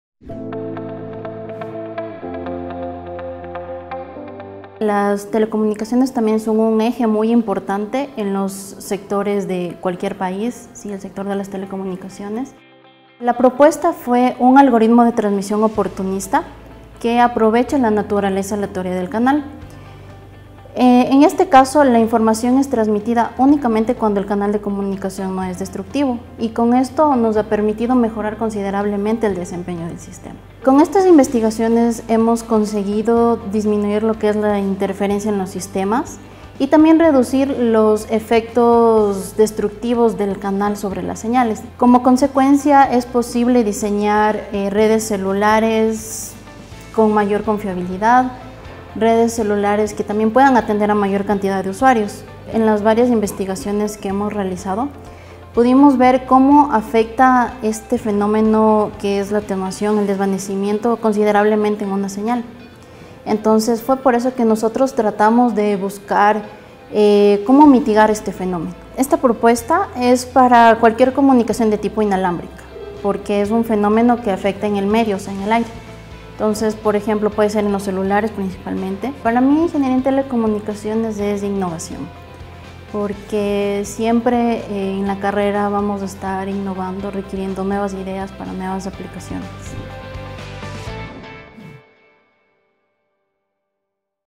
Connect 2030 ITU Membership Video: Ingeniería en Telecomunicaciones, UDLA, Ecuador
Spanish

Connect 2030 Agenda, ITU Membership contribution by Nathaly Orozco, Ingeniería en Telecomunicaciones, UDLA, Ecuador

Goal 1 – Growth: Enable and foster access to and increased use of telecommunications/ICT in support of the digital economy and society